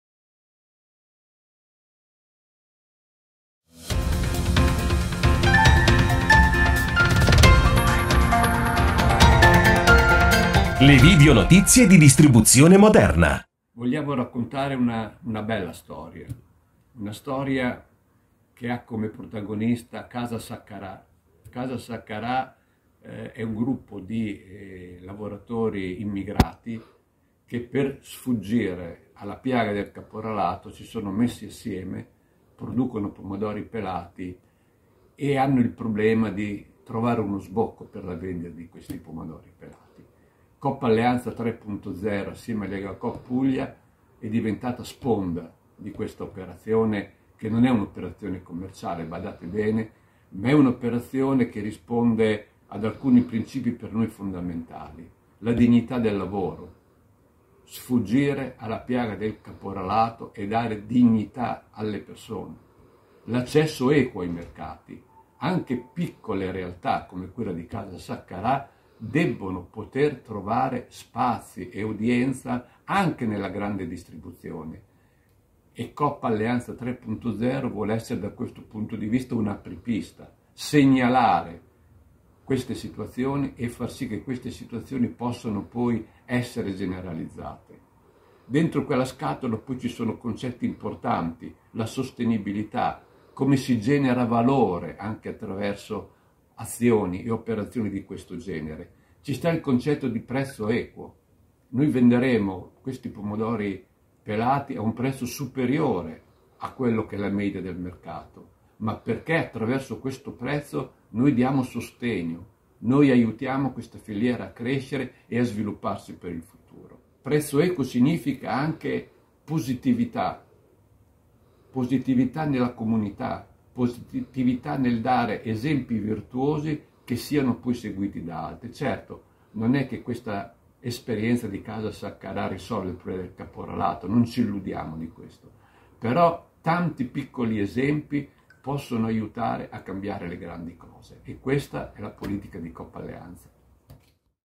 Coop Alleanza 3.0 sostiene la filiera etica
Coop Alleanza 3.0 si schiera a favore della filiera etica, sostenendo l’Associazione Ghetto - Out Casa Sankara, mettendo a disposizione dei propri clienti i pomodori pelati “Riaccolto, la terra della libertà”, prodotti dai giovani migranti fuggiti dal ghetto, che lavorano i 14 ettari di terreno gestiti dall’associazione.
L’iniziativa, promossa insieme con Legacoop Puglia, è finalizzata a sensibilizzare i consumatori sul tema dell’illegalità e del capolarato.
Ce ne parla in dettaglio Mario Cifiello, presidente Coop Alleanza 3.0.